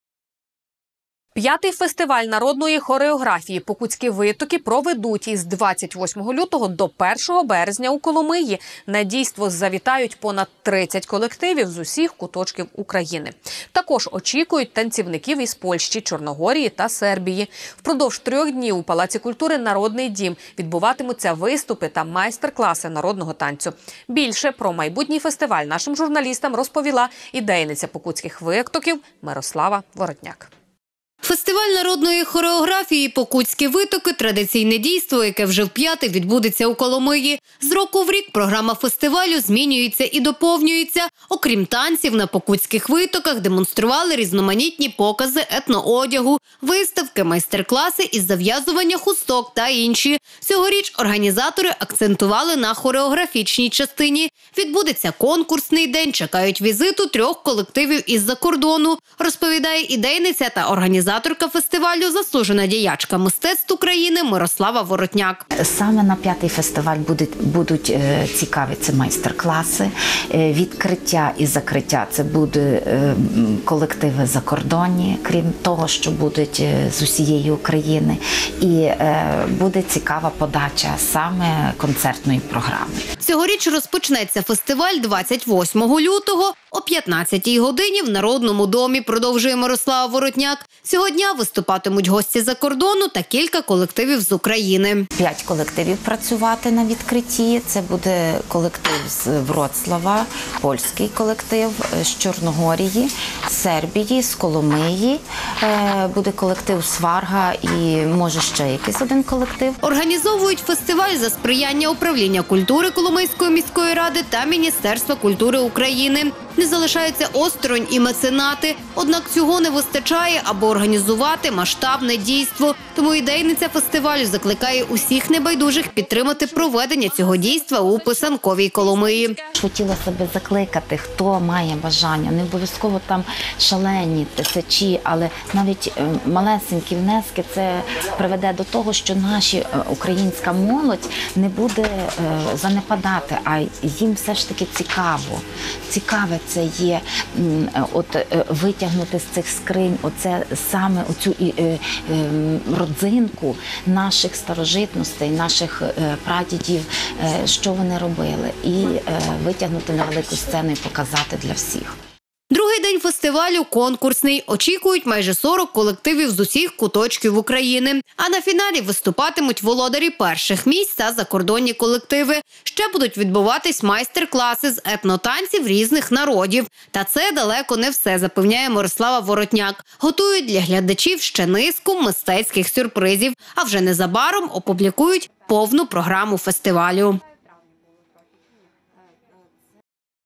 У Коломиї відбудеться фестиваль «Покутські витоки»
V фестиваль народної хореографії «Покутські витоки» відбудеться з 28 лютого до1 березня у Коломиї. На дійство завітають понад 30 колективів з усіх куточків України. Також очікують танцівників з Польщі, Чорногорії та Сербії. Впродовж трьох днів у МПК «Народний дім» відбуватимуться виступи та майстер-класи народного танцю. Більше про майбутній фестиваль нашим журналістам розповіла ідейниця «Покутських витоків» Мирослава Воротняк.